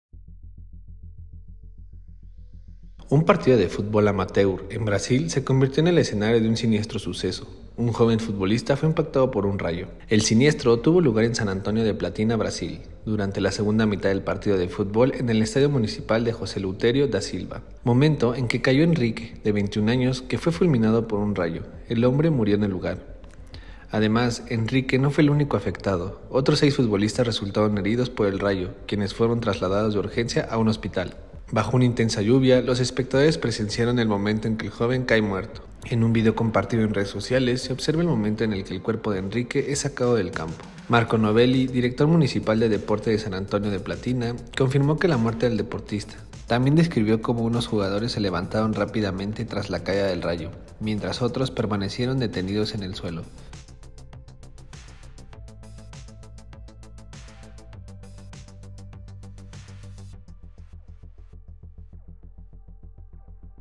Futbolista muere en partido amateur tras ser alcanzado por un rayo
Un partido de futbol amateur en Brasil se convirtió en el escenario de un siniestro suceso: un joven futbolista fue impactado por un rayo.

El siniestro tuvo lugar en San Antonio de Platina, Brasil, durante la segunda mitad del partido de futbol en el estadio municipal José Eleutério da Silva, momento en que Caio Henrique, de 21 años fue fulminado por un rayo. El hombre murió en lugar.